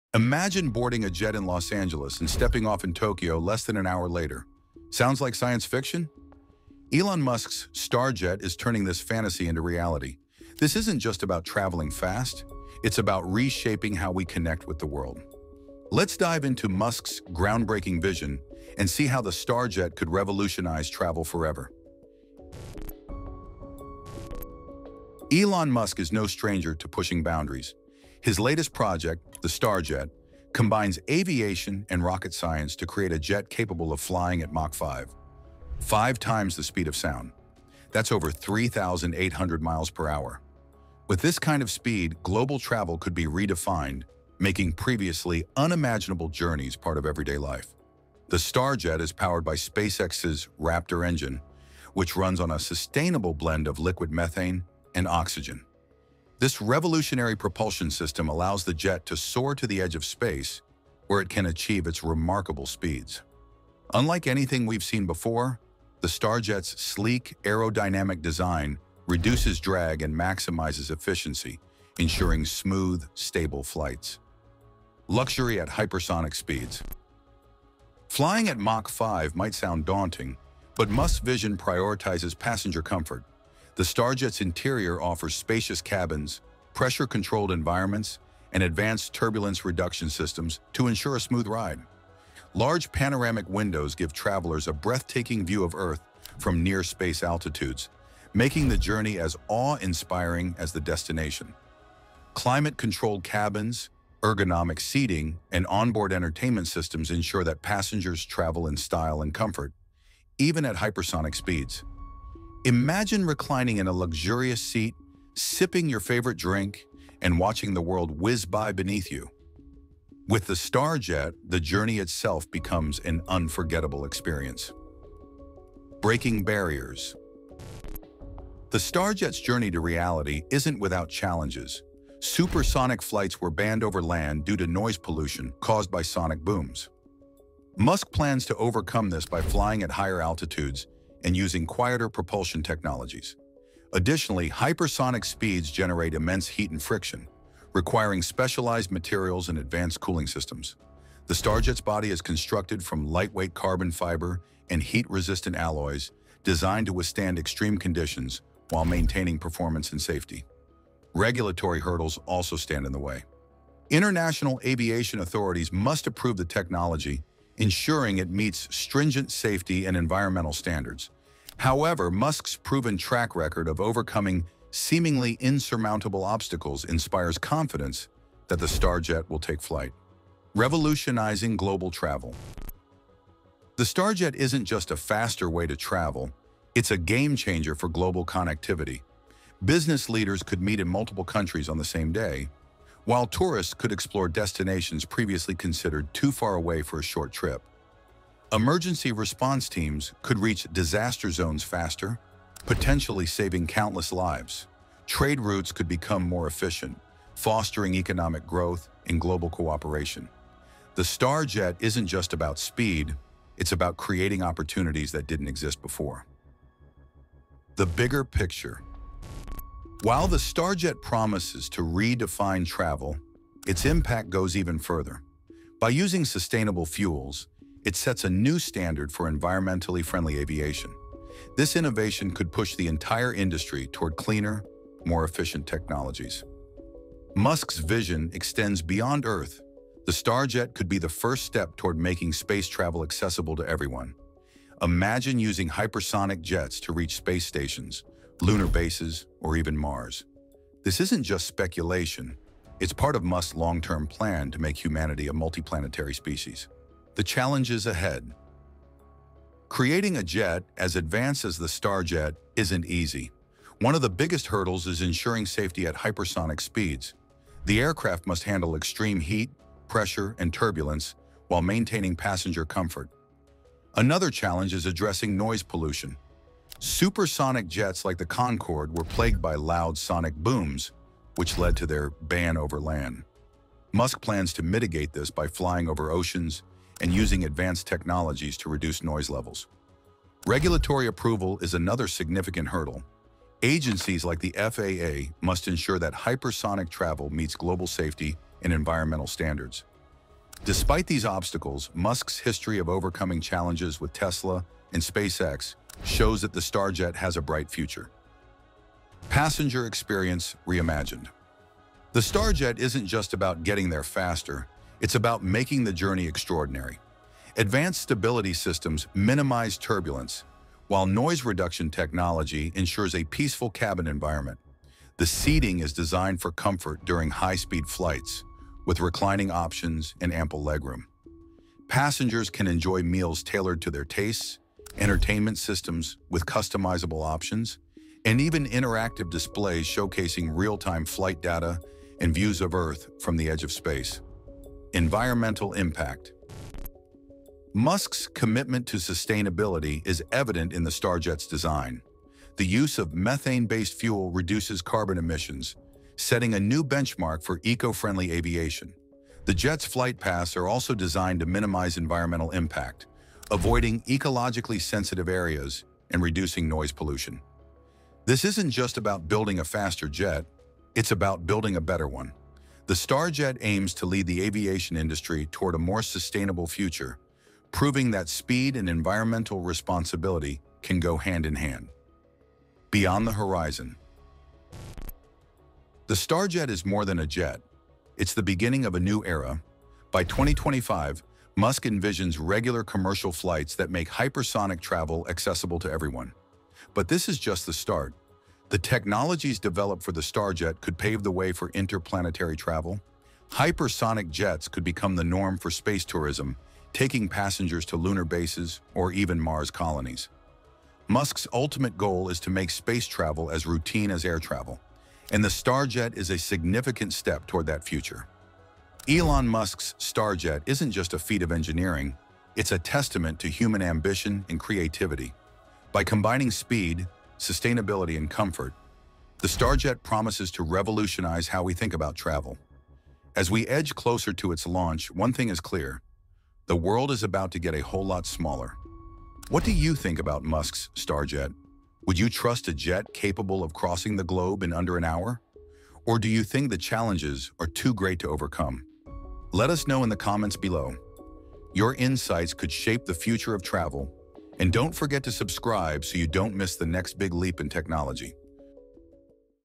Elon Musk’s Revolutionary Supersonic Space Jet Shocks the Public
Elon Musk's revolutionary supersonic space jet has shocked the public with its groundbreaking technology. Learn all about this game-changing innovation in this video!